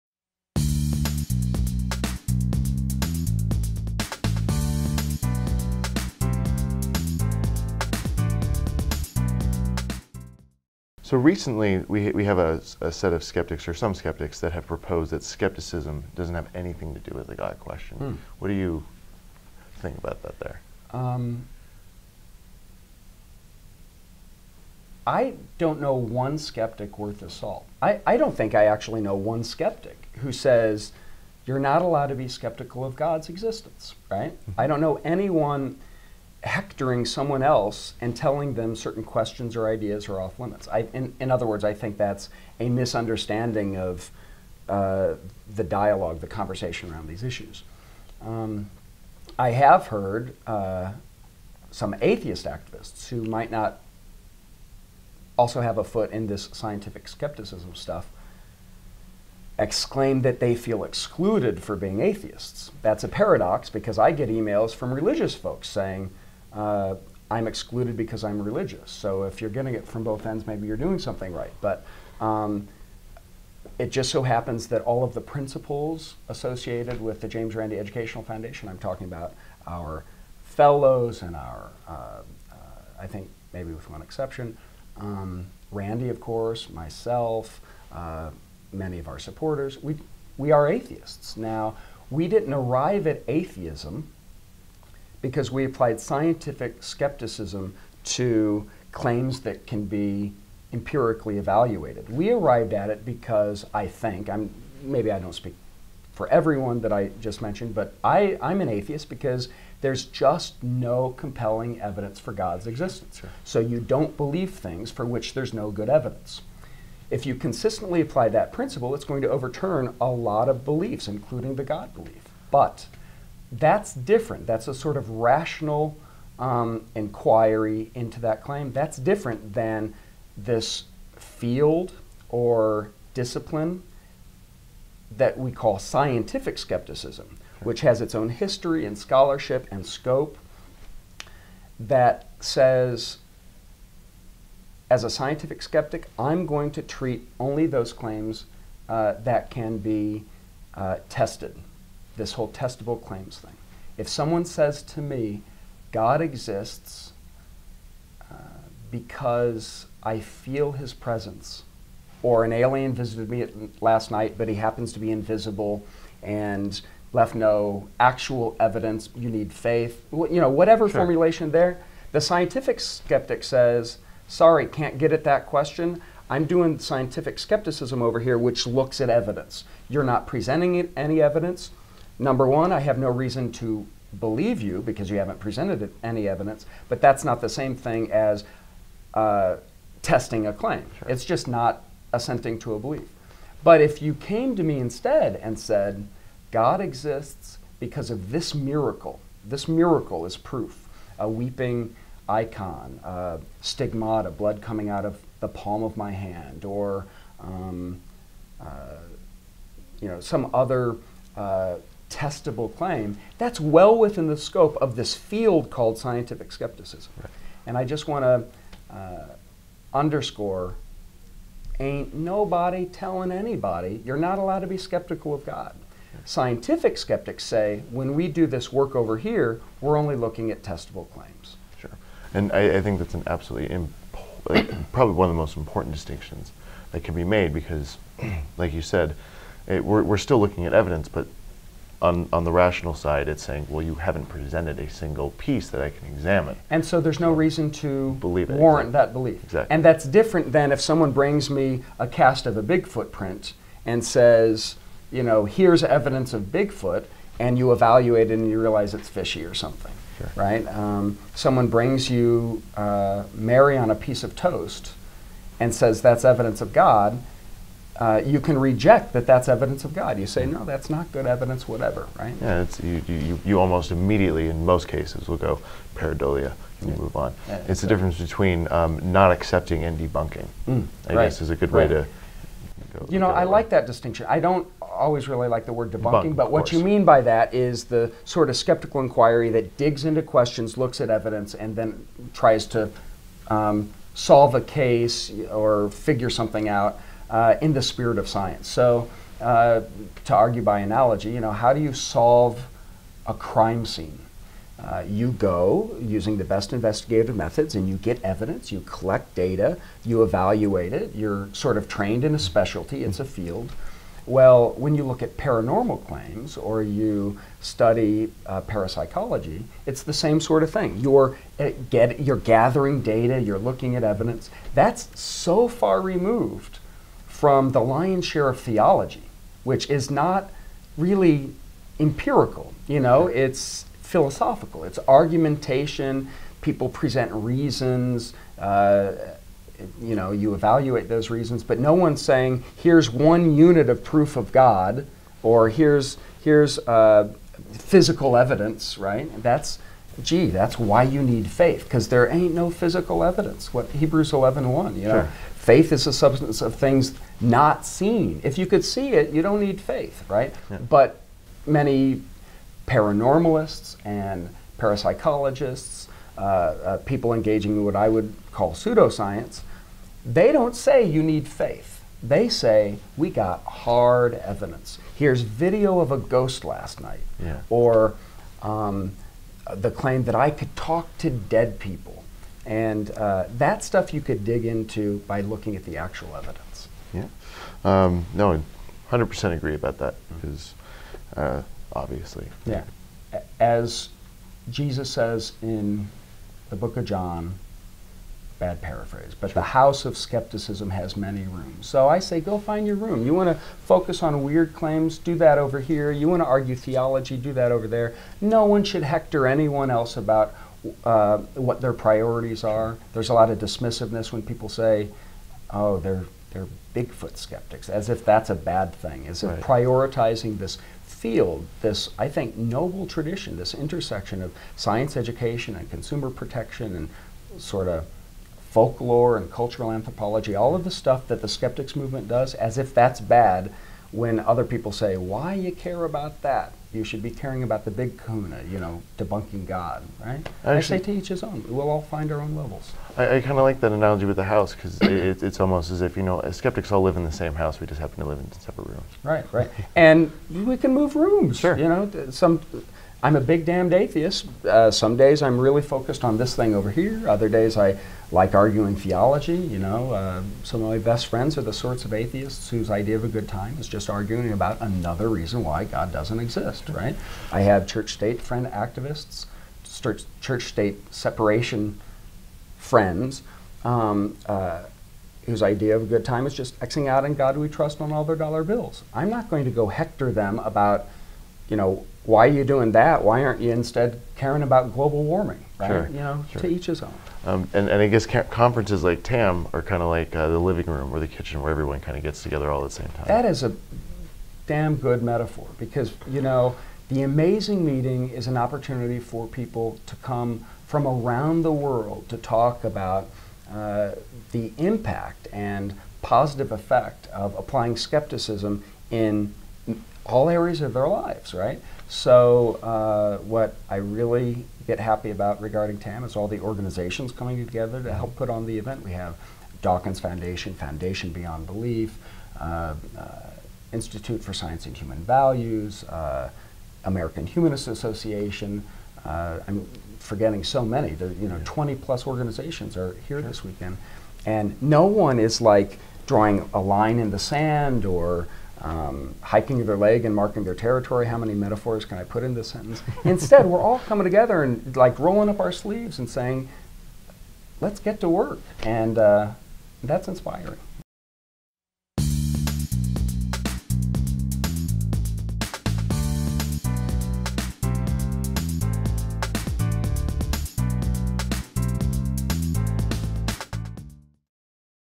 Meet the Amazing TAMers: DJ Grothe Part 2
Joel Guttormson, Outreach and Event Coordinator for the Richard Dawkins Foundation for Reason and Science interviews speakers from TAM 2013 presented by the James Randi Educational Foundation.

D.J. Grothe- He is President of the James Randi Educational Foundation.  He has lectured widely on topics at the intersection of education, science and belief at universities such as Stanford, Harvard, Yale, UCLA, UC Berkeley, and dozens of others. Formerly a professional magician and mentalist, he has special interests in the psychology of belief and processes of deception and self-deception. He currently hosts the radio show and podcast For Good Reason.

Produced by the Richard Dawkins Foundation for Reason and Science